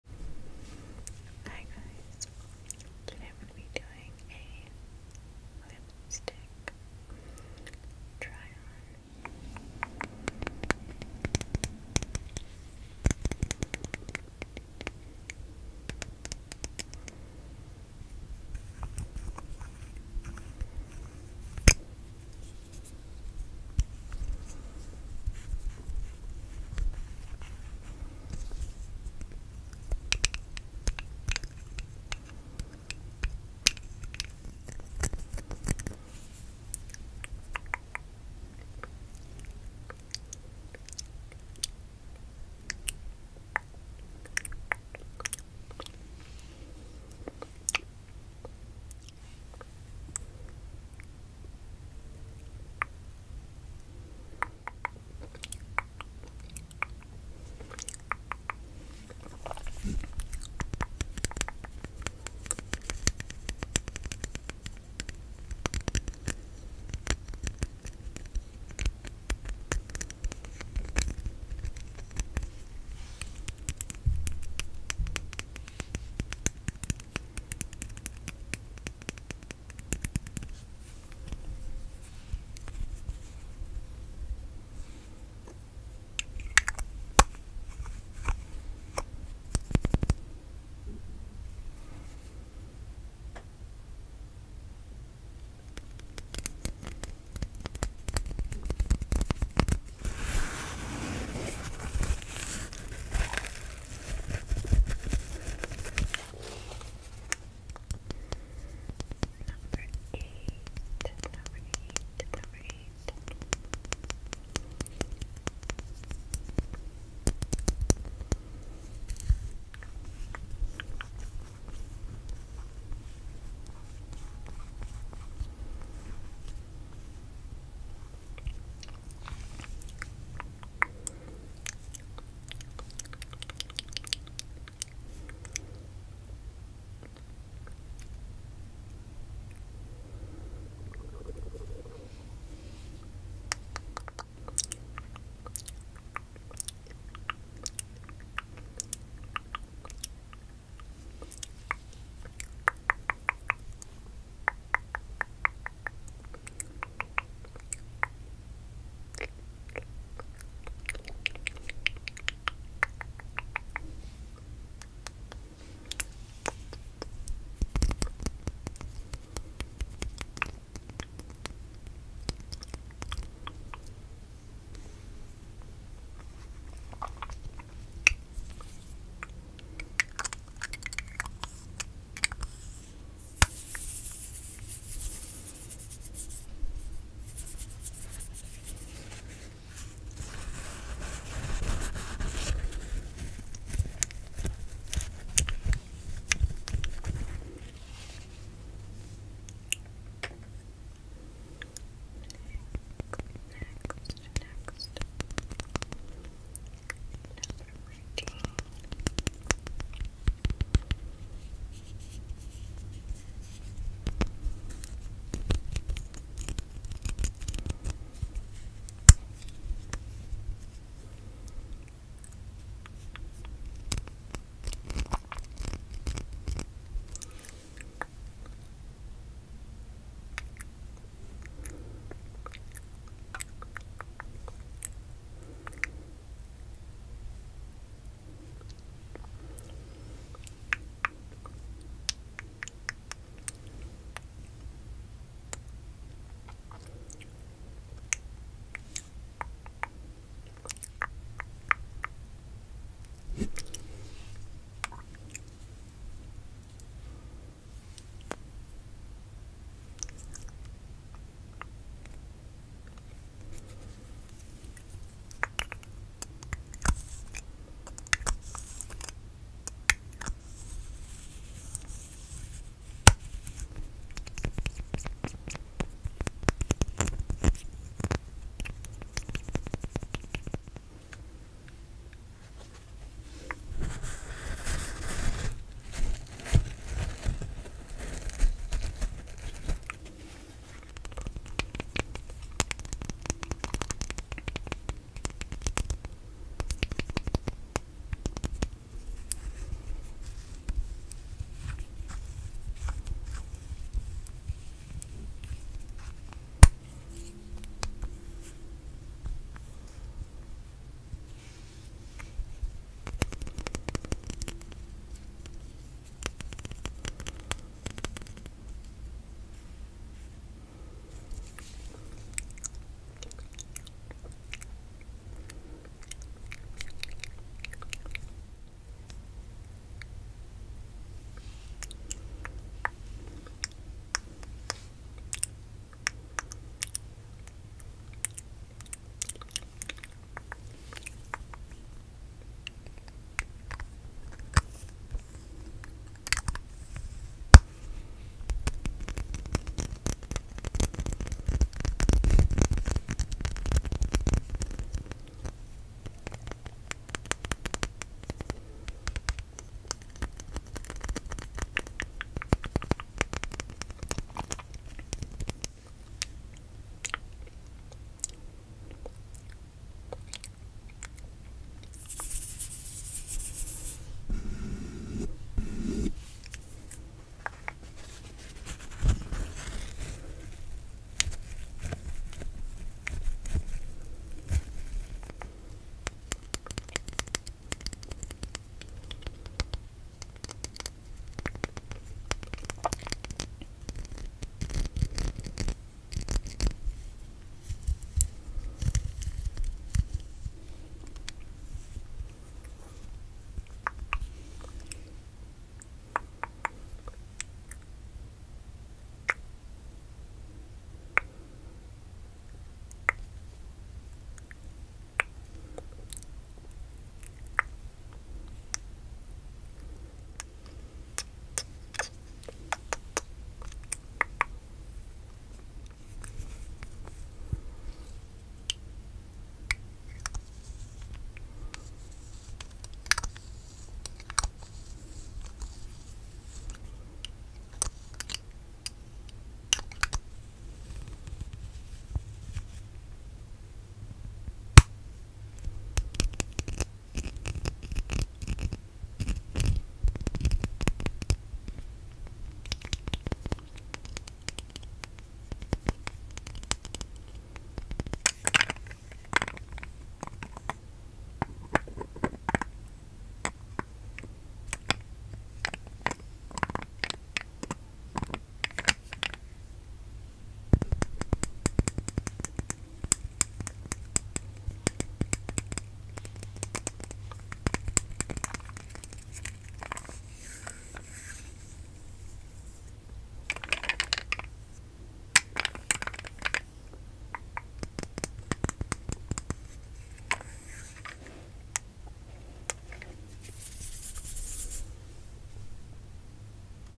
Asmr Lipstick Try On Whispering
Hello, welcome to my asmr channel! Todays video is a lipstick try on! Hope your enjoy!